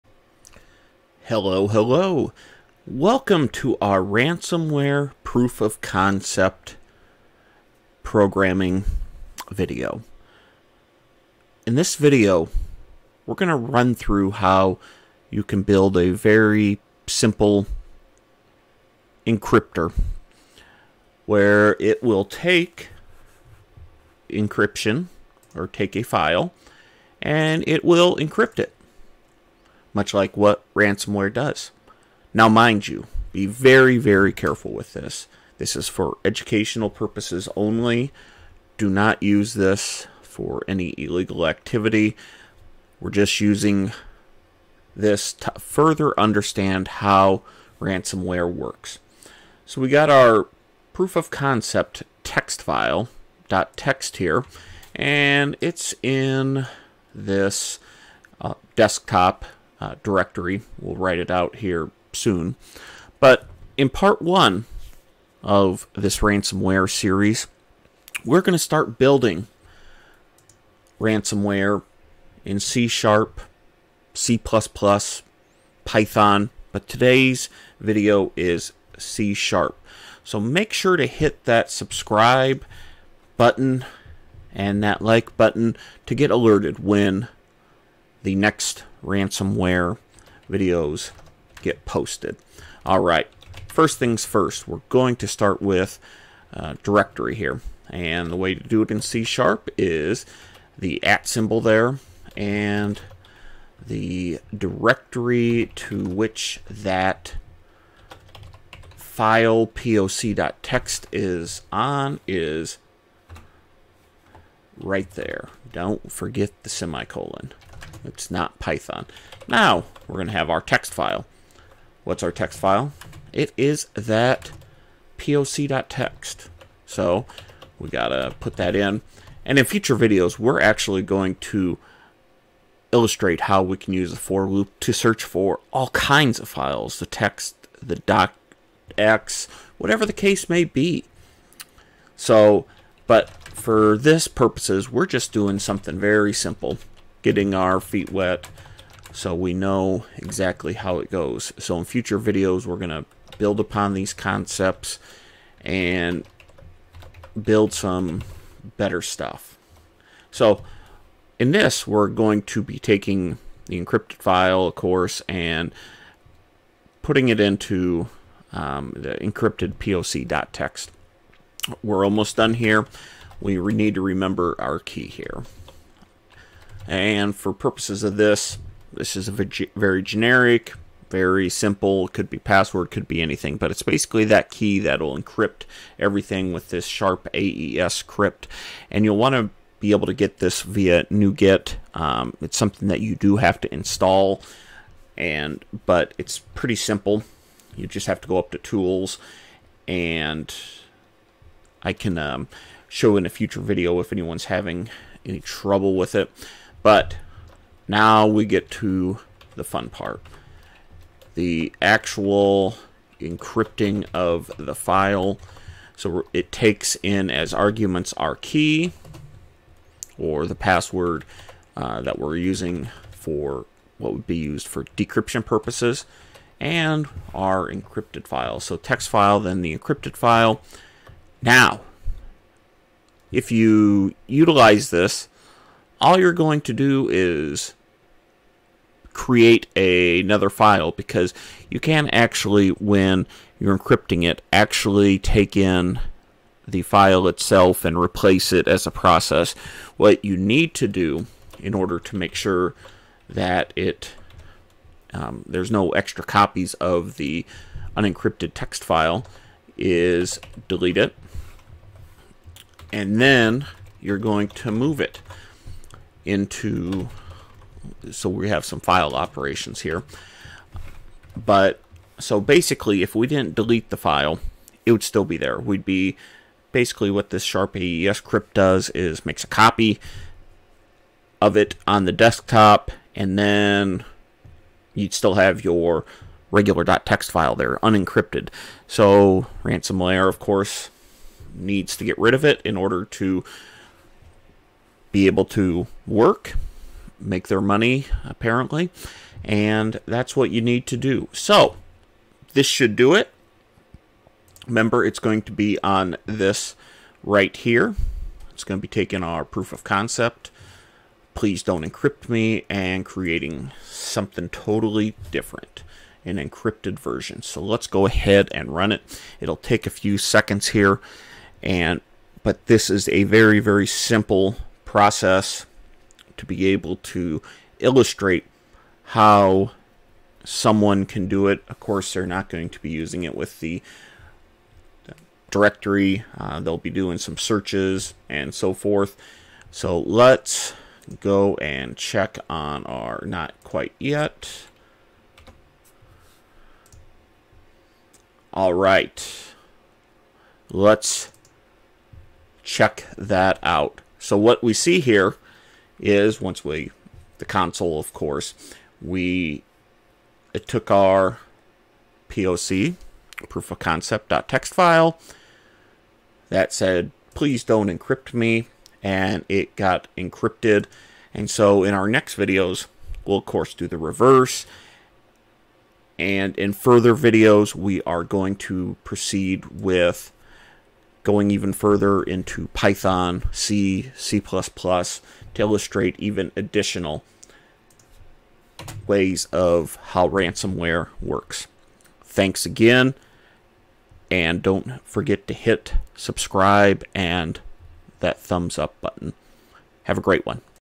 Building a File Encrypter to Understand Ransomware Attacks
Ever wonder how ransomware actually locks up files on a system? In this video, we strip away the mystery and write a functional file encrypter from scratch using C#.

By understanding how malware traverses directories and interacts with the Windows API to lock files, security professionals and developers can build better detection tools and understand the mechanics of a ransomware attack.

We will be using the .NET framework and System.Security.Cryptography to build a tool that recursively finds files and secures them with AES encryption.